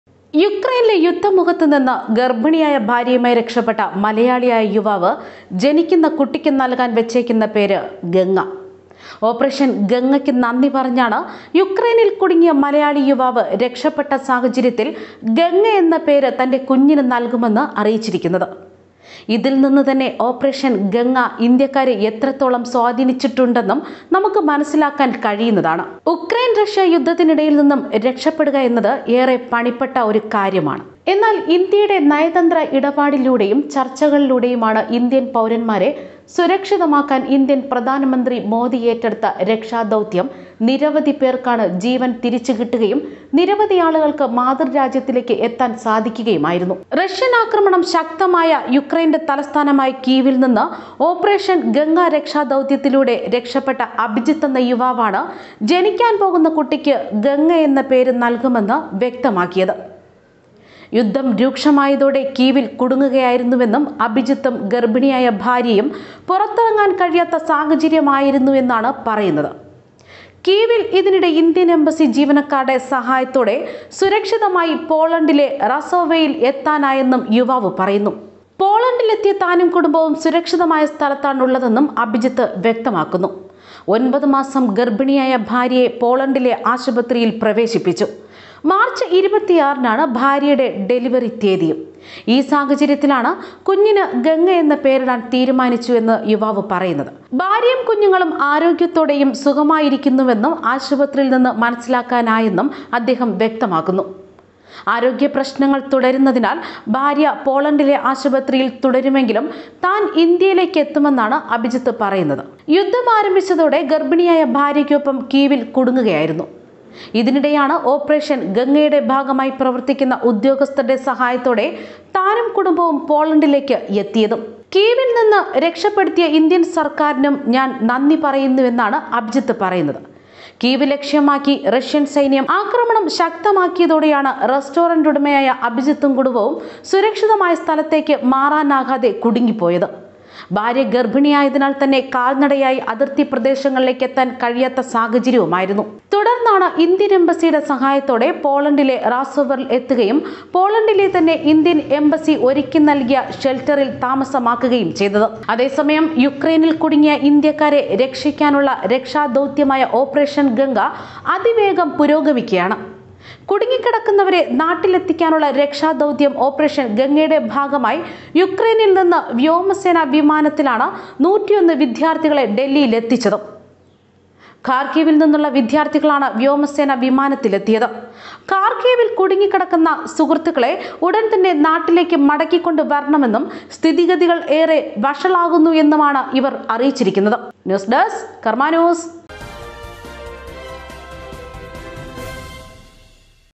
ഓപ്പറേഷൻ ഗംഗ രക്ഷപ്പെട്ട മലയാളിയായ യുവാവ് , കുഞ്ഞിന് നൽകുന്ന പേര് "ഗംഗ"
യുക്രൈനിലെ  യുദ്ധമുഖത്തു നിന്ന്  ഗർഭിണിയായ ഭാര്യയുമായി രക്ഷപെട്ട  മലയാളിയായ യുവാവ് , ജനിക്കുന്ന കുട്ടിക്ക്  നല്കാൻ  വച്ചേക്കുന്ന പേര് ഗംഗ ,ഓപ്പറേഷൻ ഗംഗ'യ്ക്ക് നന്ദി പറഞ്ഞ് ആണ് യുക്രൈനിൽ കുടുങ്ങിയ മലയാളി